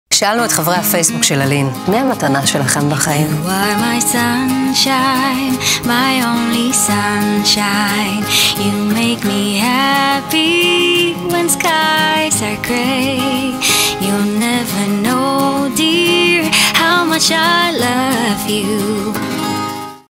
מי המתנה שלכם בחיים?
שאלנו את חברי הפייבסוק של ללין, מי המתנה שלהם בחיים